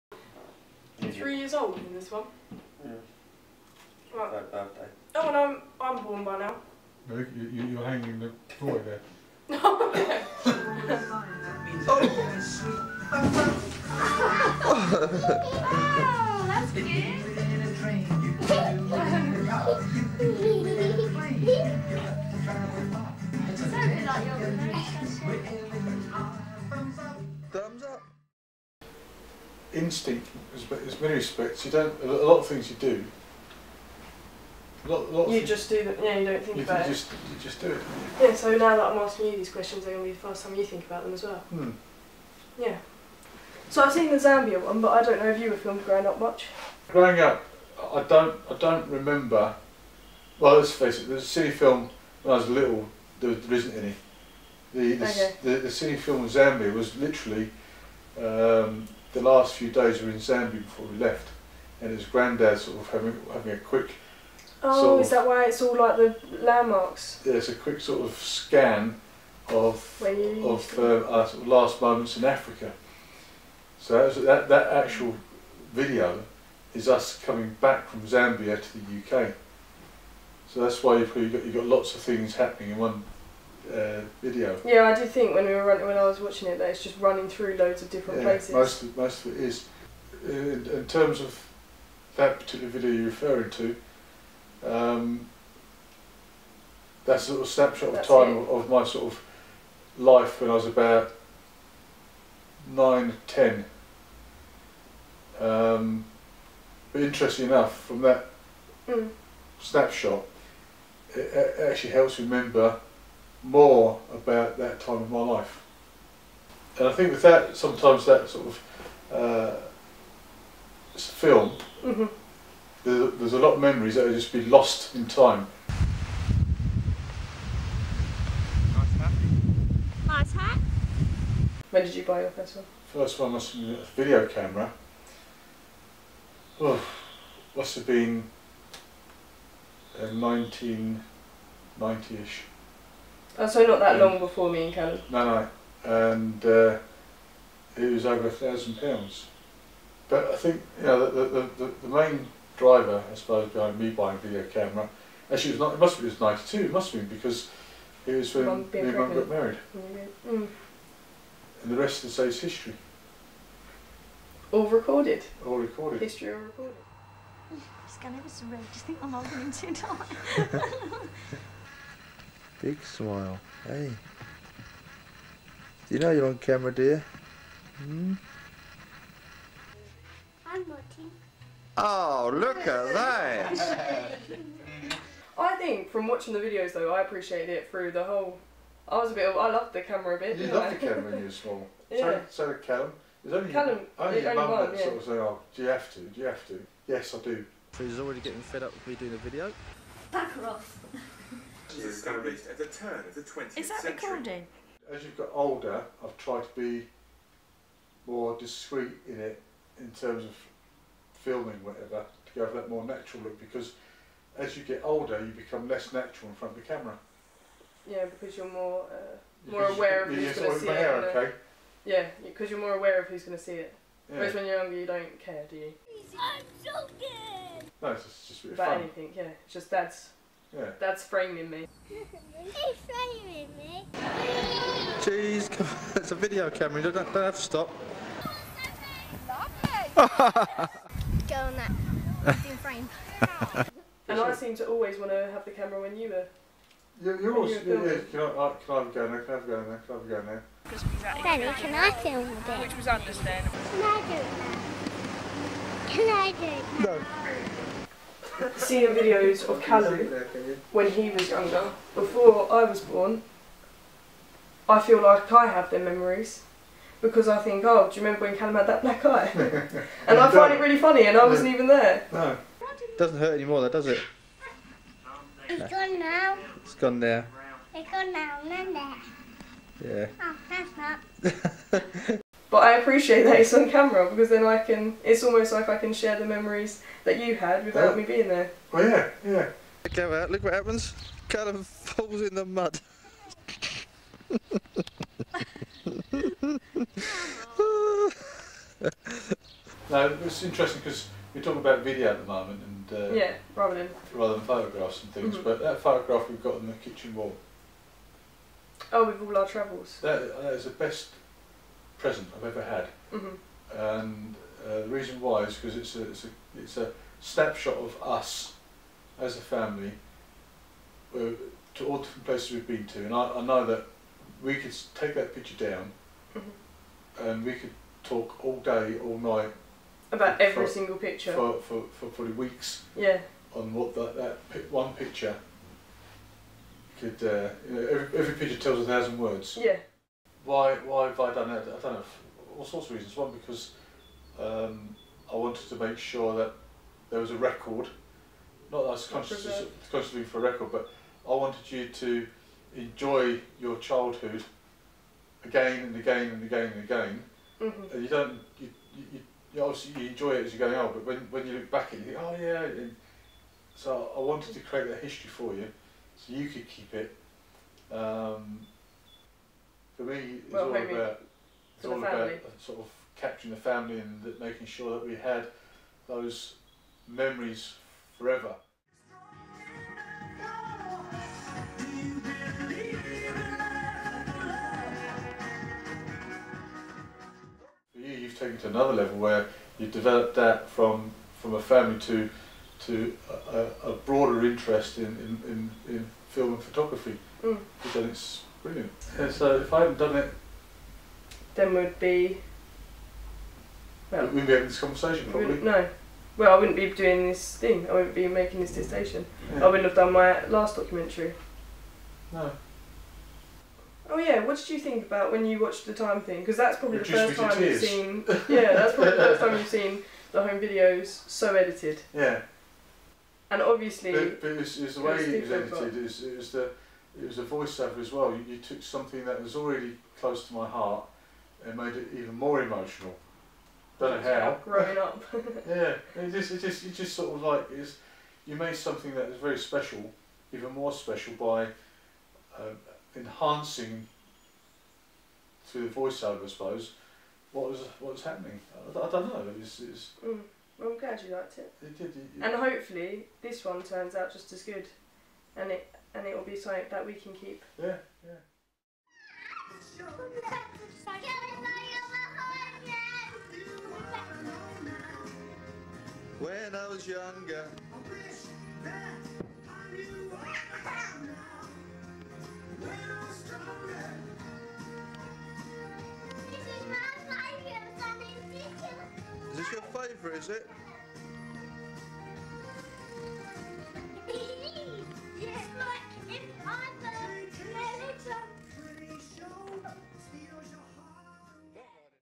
Framing Me - A Documentary
A documentary I made to go toward my final year creative dissertation, completed at Brighton University.
The film shows my home videos played as a visual narrative alongside a conversation with me and my father talking about our relationship with the camera over the years.